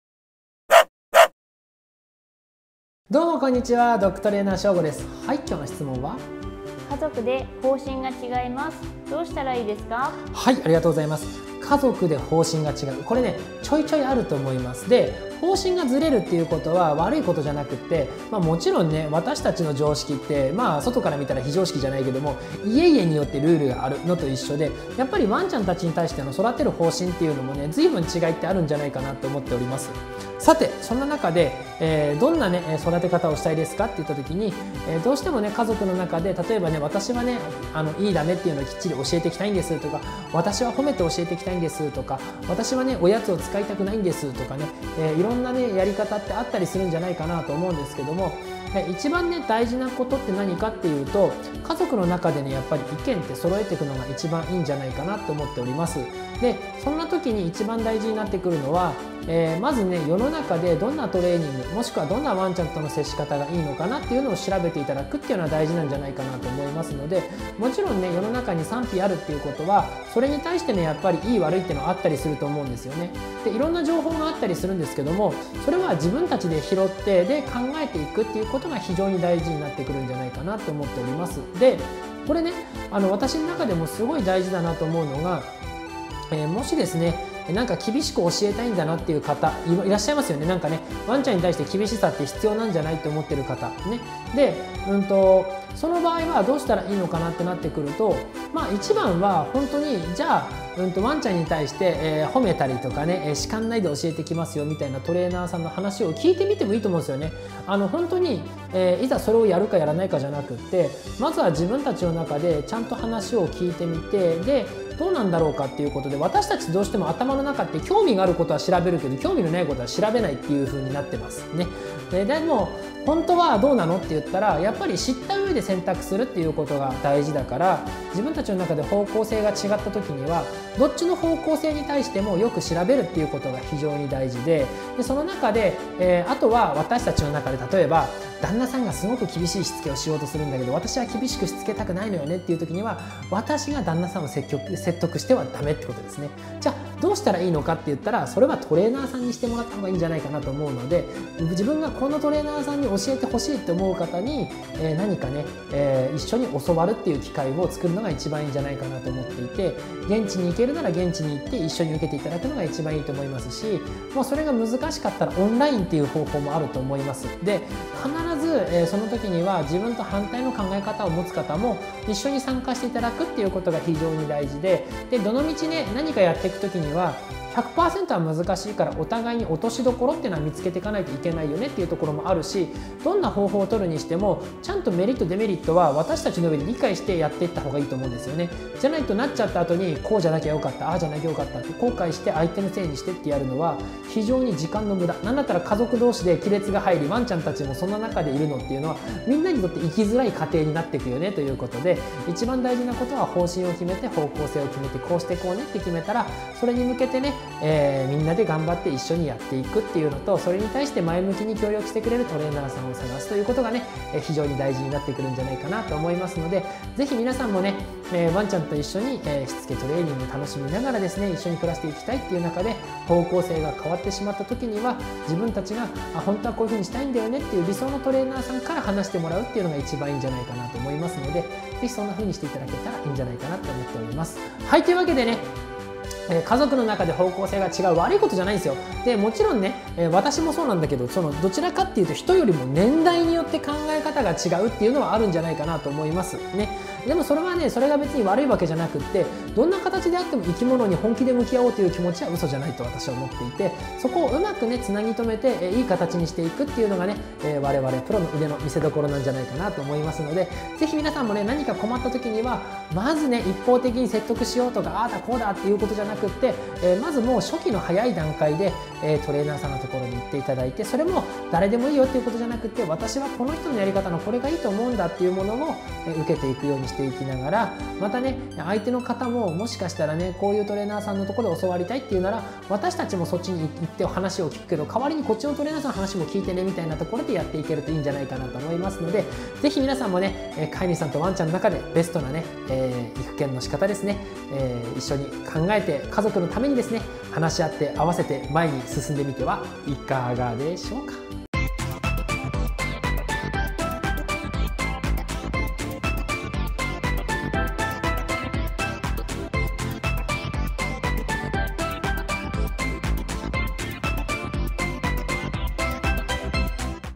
家族で方針が違う時どうすればいい？
今回は現役ドックトレーナーによる愛犬のお悩み解決コーナーです^^
こちらの動画も参考にしてみて下さい。

●柴犬②

【関連動画】